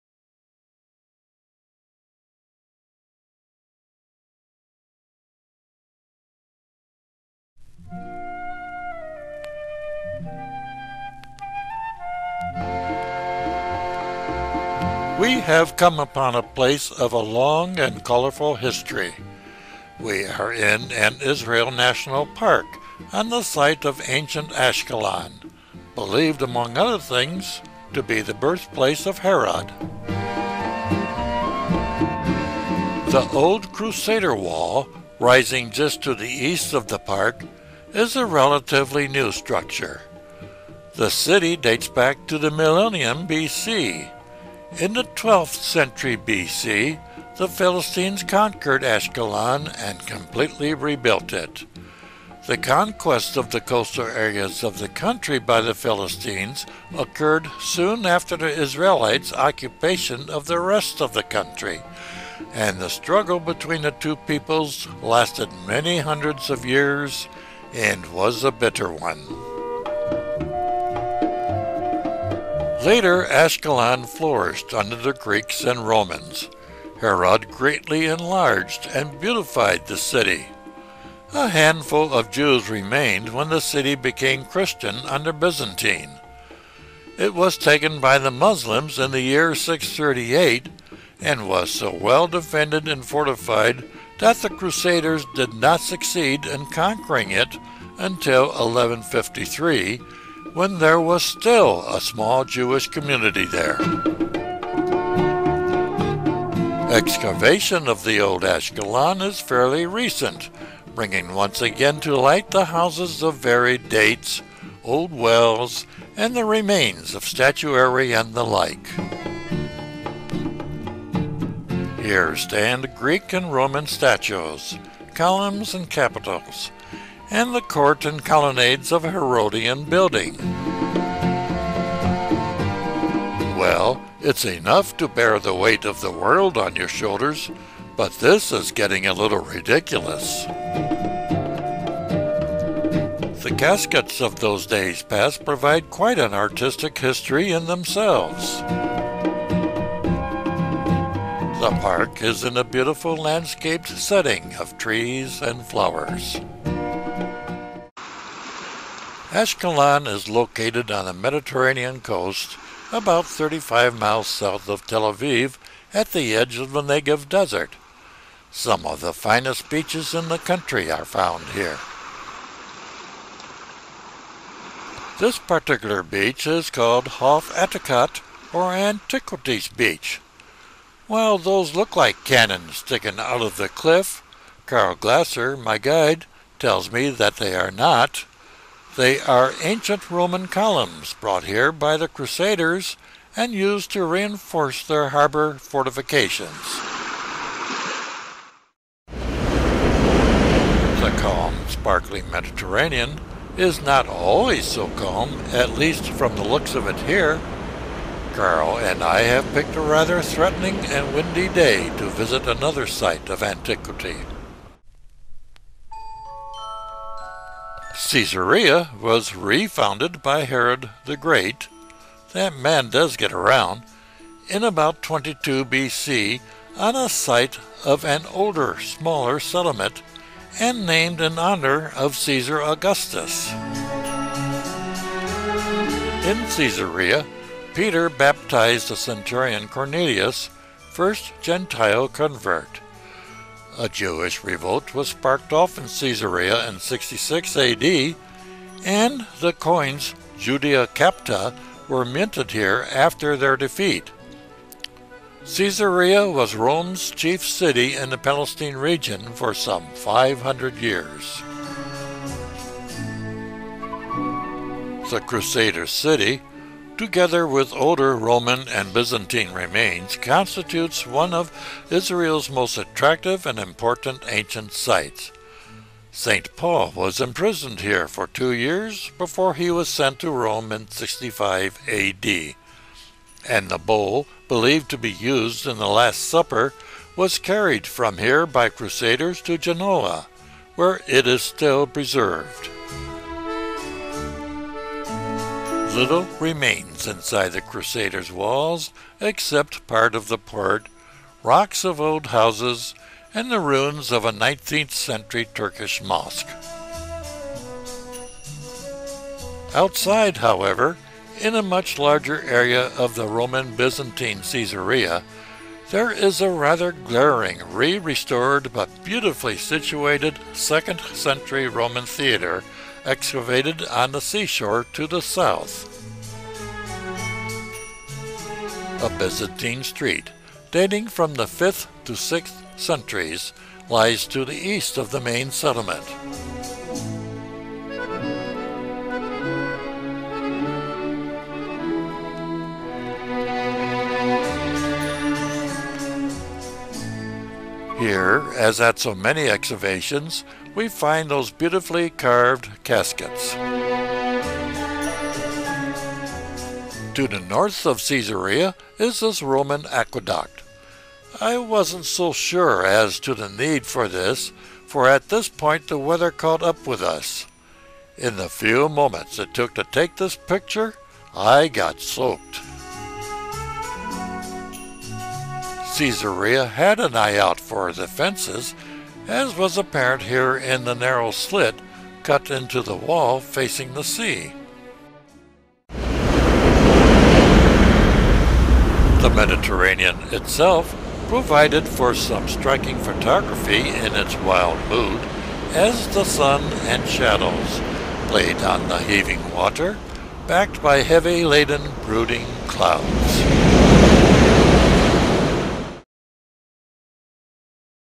Antiquities of Ashkelon and Caesarea
Narrated slide show video from "My Vacation in Israel - 1971".  Excavations of statuary, caskets, etc. from the ancient cities of Ashkelon and Caesarea along Israel's Mediterranean coast.